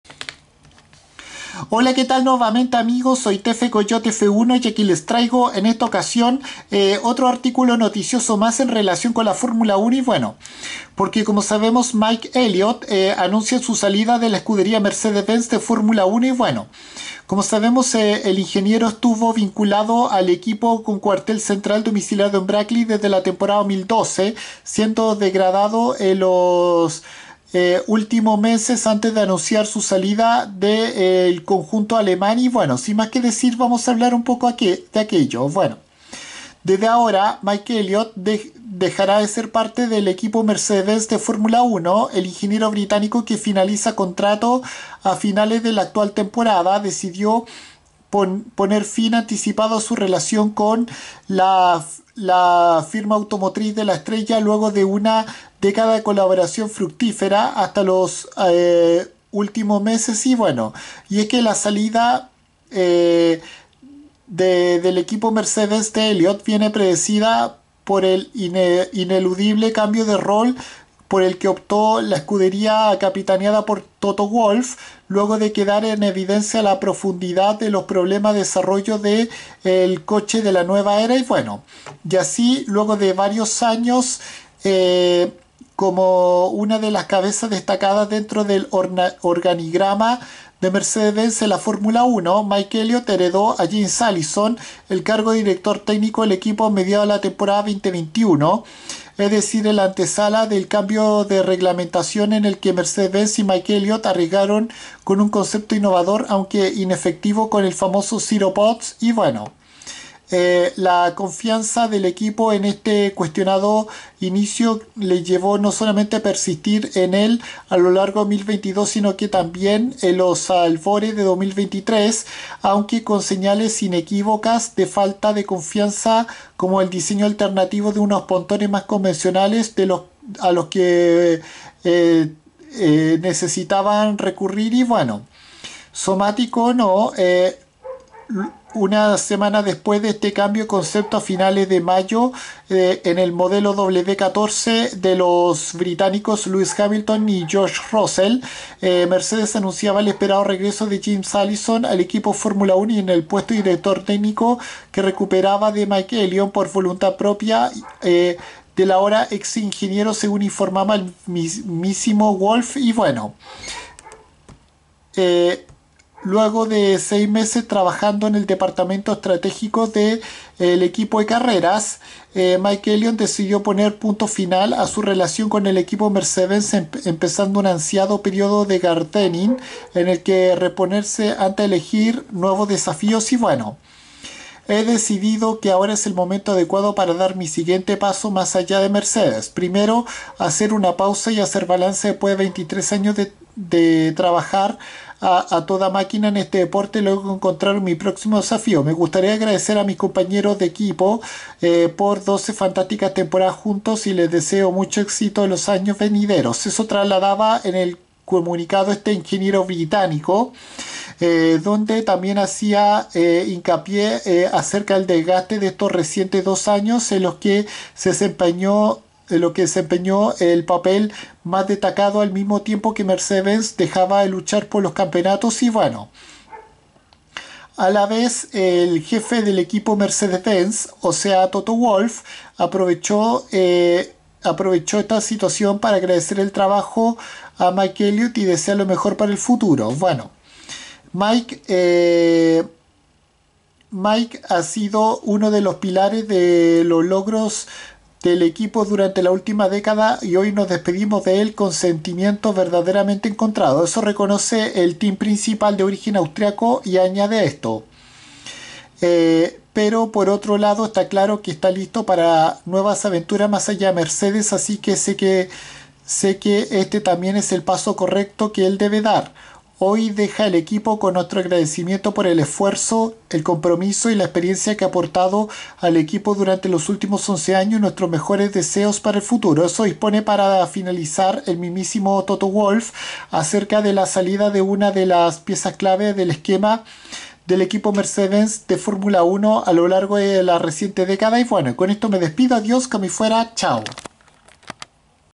Mike Elliott se retira del equipo Mercedes Benz de Fórmula 1
Luego de 11 años el ingeniero británico ya no será parte del conjunto alemán de la estrella